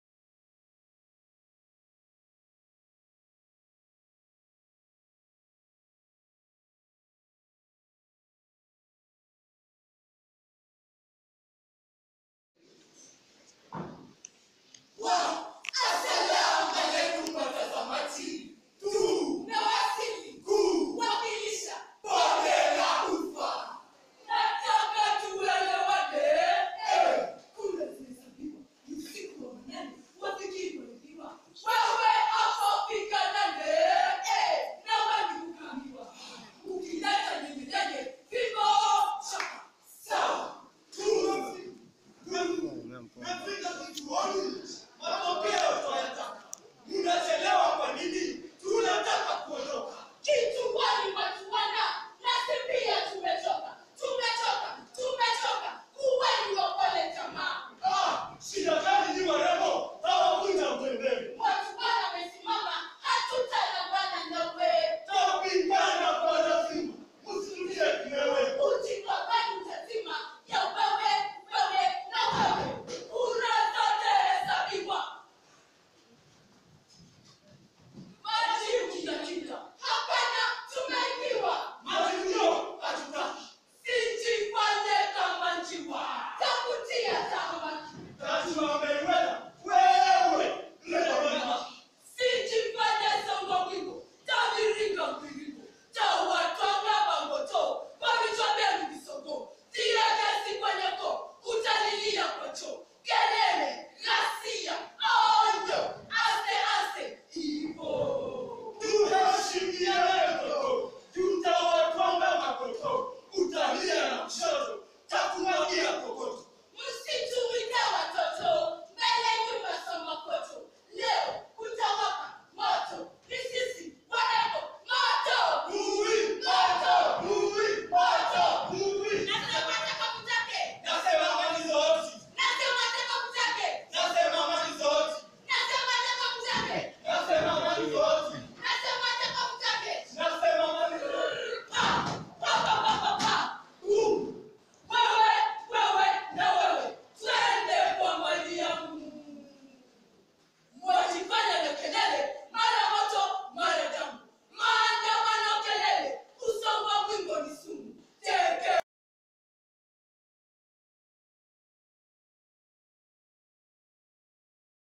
DRAMATIZED VERSE- MANIZOHOTI 2018 EGERTON |KENYA UNIVERSITIES' PERFORMANCES, ARTS AND FILM FESTIVALS
TITLE: USONGOMBWINGO || MANIZOHOTI
SCRIPTED BY GEORGE MIKE MOKA (MOKATHEPOET)
DIRECTED BY GEORGE MIKE MOKA (MOKATHEPOET)

SYNOPSIS

Jukwani ni Usongombwingo, sarakasi kati ya wanafunzi wanaofanya uchaguzi na matumaini ya kupata Ushindi, ilhali wanageuka na kusema kuwa matokeo yameibiwa. Wanazua fuja vurugu faraghani basi Polisi wanaingilia kati. Burudika.

WAHUSIKA
1. Polisi || Wasimamizi wa Uchaguzi
2. Wanafunzi wa Kike ||Wafuasi wa Jackline Tekofu (Tekofu Jake)
3. Wanafunzi wa Kiume || Wafuasi wa Manuel Zihoti (Manizohoti)

CAST.
- Damaris Njeri
- Gloria Amwayi
- Esther Nyamoita
- Juju Kajuju
- Mercy Rono
- Lavenda Bigfish
- Evey Mwangi
- Mush BM Wangari
- Vashty Babu
- Irene Kiambati
- Joe Ochoro
- Brian Otieno
- Kodila Zablon

- Isaac Kiluta
(c)EGERTON DRAMA CLUB 2018

BUY GOODS TILL NUMBER 5231009